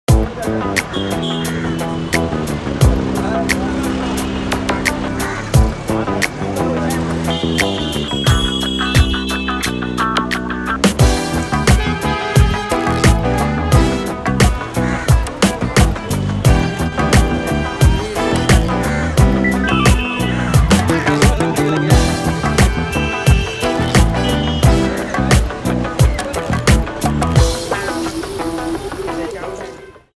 Tea shop, Kolkata, Calcutta, West Bengal
This video features a teashop in Kolkata (Calcutta), West Bengal state of India.